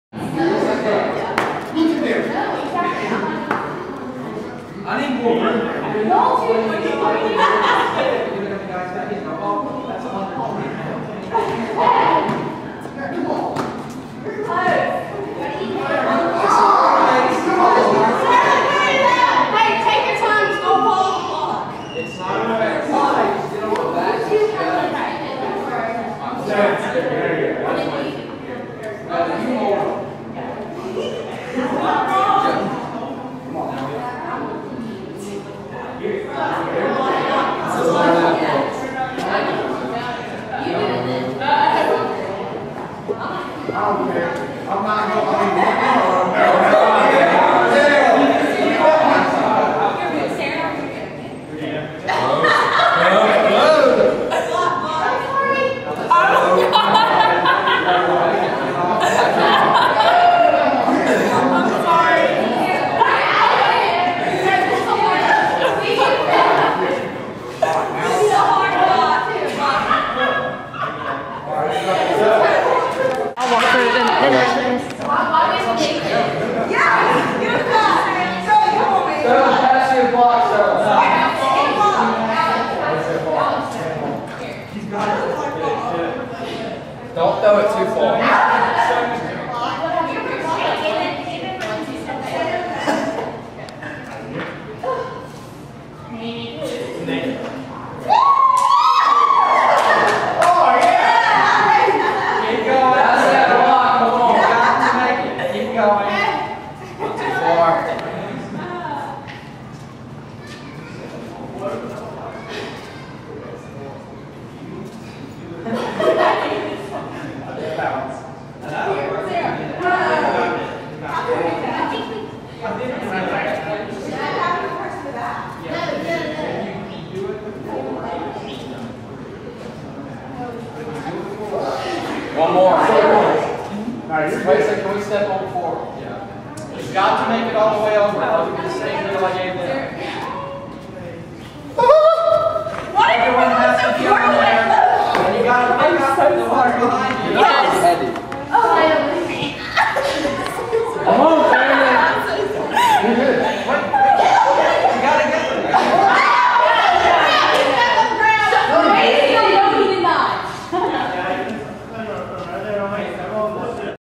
Sociology Project
Sociology students formed teams and participated in an experiential project that involved moving between two fixed stations (mats) using only a set number of stepping stones.   The students had to make their way using only the stepping stones without touching the ground or any other solid surface.  To complete the task, the whole group had to arrive on the other side and retrieve the stepping stones.